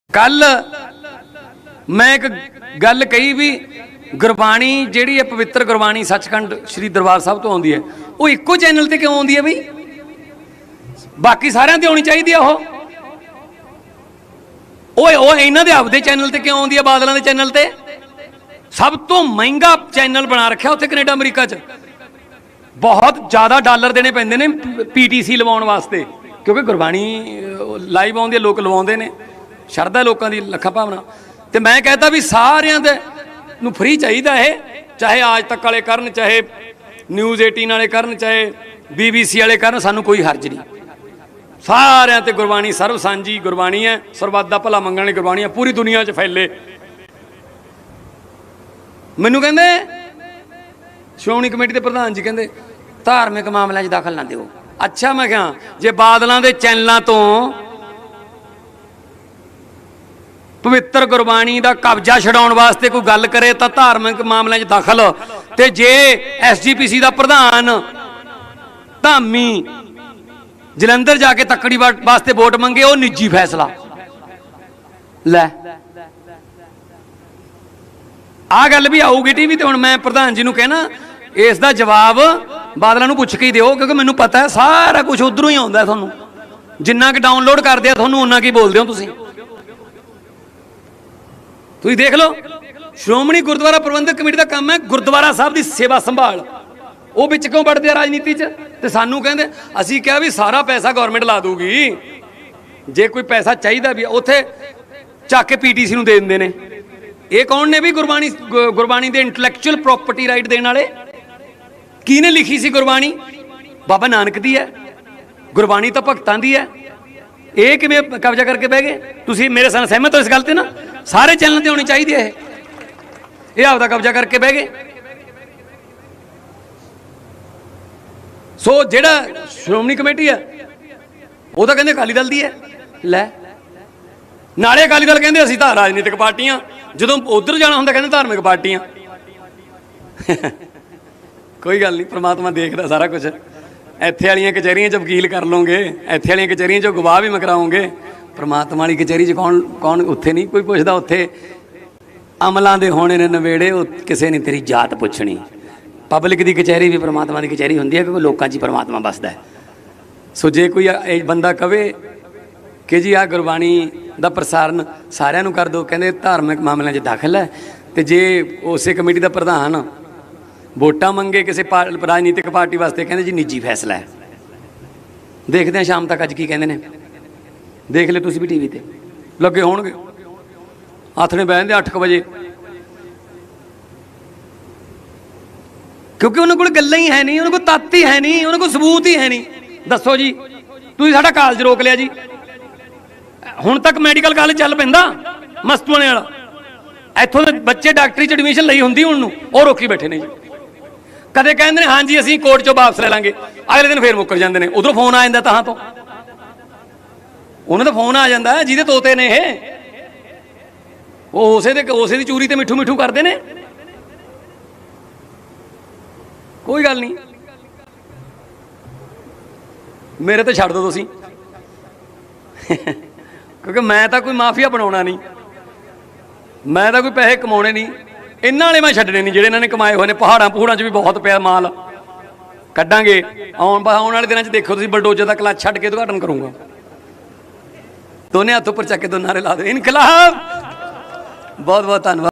ਸੀ ਐੱਮ ਭਗਵੰਤ ਮਾਨ ਨੇ ਫੇਰ ਛੇੜੀ sgpc ਦੀ ਗੱਲ, ਜਿਨਾਂ ਦੇ ਤੋਤੇ ਨੇ,ਓਸੇ ਦੀ ਚੂਰੀ ਤੇ ਮਿੱਠੂ ਮਿੱਠੂ ਕਰਦੇ ਨੇ।
ਸੀ ਐੱਮ ਭਗਵੰਤ ਮਾਨ ਨੇ ਫੇਰ ਛੇੜੀ sgpc ਦੀ ਗੱਲ,
ਜਿਨਾਂ ਦੇ ਤੋਤੇ ਨੇ,ਓਸੇ ਦੀ ਚੂਰੀ ਤੇ ਮਿੱਠੂ ਮਿੱਠੂ ਕਰਦੇ ਨੇ।